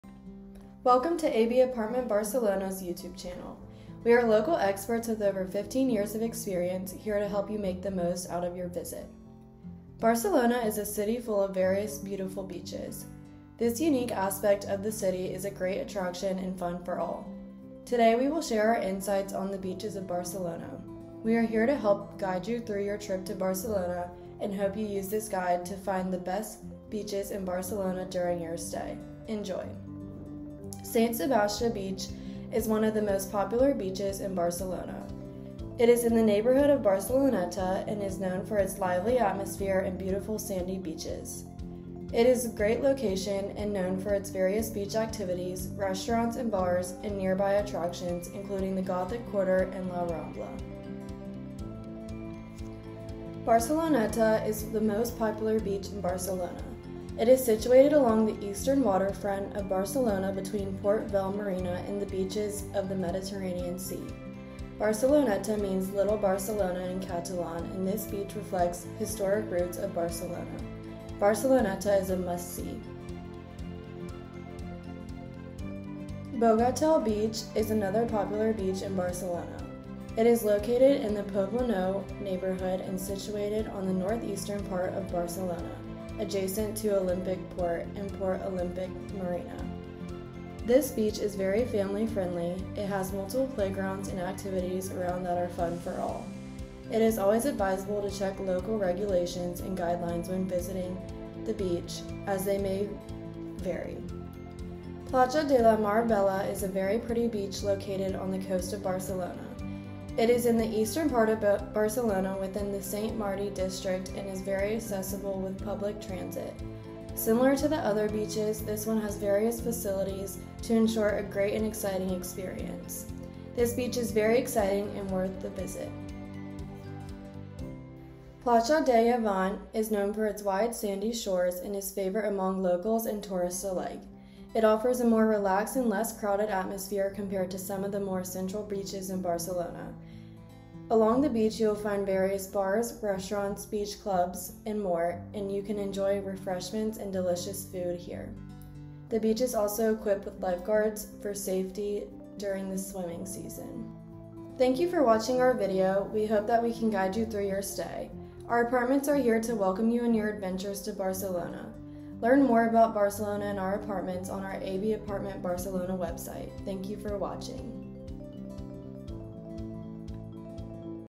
The Best Beaches in Barcelona
Barcelona is full of beautiful beaches that are unique in their own way. There are so many beaches to explore, but today we are going to show the five best beaches in the city of Barcelona.

We have made a complete travel guide to beaches in Barcelona for this summer. On this guide, you will find the location, see the views, and take a quick trip to these five beaches. Lots of questions can come up about which beach to visit in barcelona. We are going to cover everthing you need to know about these amazing beaches!

We at AB Apartment Barcelona have created a video to go more in-depth about guiding you through your travels to the beaches in Barcelona.